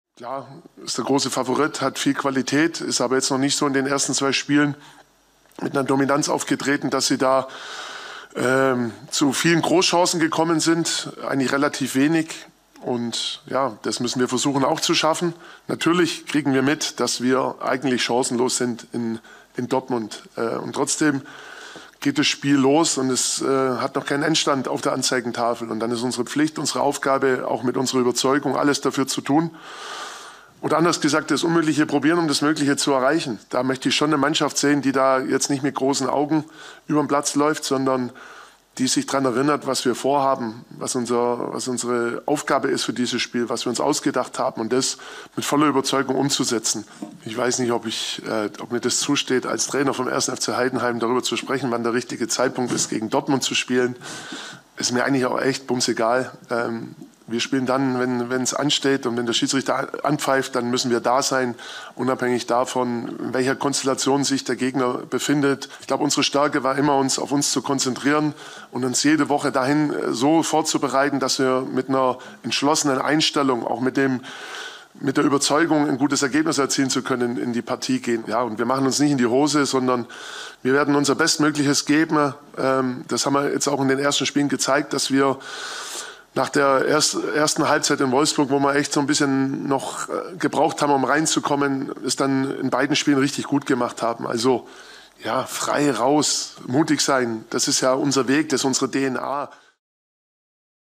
Heidenheim zu Gast beim BVB: "Machen uns nicht in die Hose" | SID
Aufsteiger 1. FC Heidenheim will seine schwierige Aufgabe bei Borussia Dortmund mit dem nötigen Respekt, aber auch mit viel Mut angehen. "Wir machen uns nicht in die Hose", sagte Trainer Frank Schmidt vor dem Bundesliga-Auswärtsspiel beim BVB.